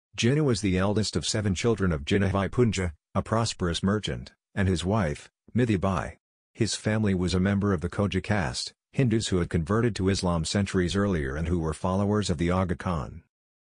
About Muhammad Ali Jinnah Baba e qoum
Jinnah was the eldest of seven children of Jinnahbhai Poonja, a prosperous merchant, and his wife, Mithibai. His family was a member of the Khoja caste, Hindus who had converted to Islam centuries earlier and who were followers of the Aga Khan.